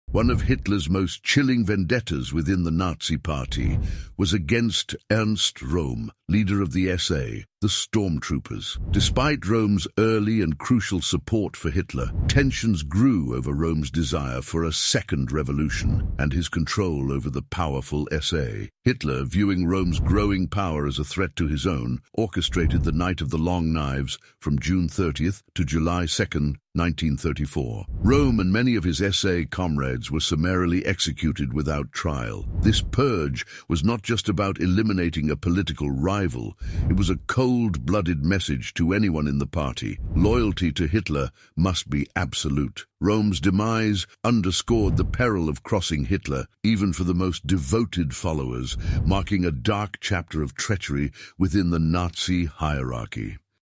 Hitler's Vendetta: The Downfall of Ernst Rohm in 60 Seconds
Explore one of the most chilling internal power struggles of the Nazi Party in this YouTube Short, focusing on Hitler's vendetta against Ernst Rohm, the leader of the SA (Sturmabteilung). Discover the tumultuous relationship between Hitler and Rohm, culminating in the Night of the Long Knives, where Rohm and other SA leaders were brutally purged under Hitler's direct orders. In just 60 seconds, learn about the reasons behind this ruthless action, its execution, and the significant impact it had on consolidating Hitler's power within the Nazi Party and Germany. This video provides a quick but deep look into the deadly political intrigues of the Third Reich, illustrating the lengths to which Hitler would go to eliminate threats to his authority. Join us for a brief journey into one of the Nazi regime's most pivotal and murderous moments.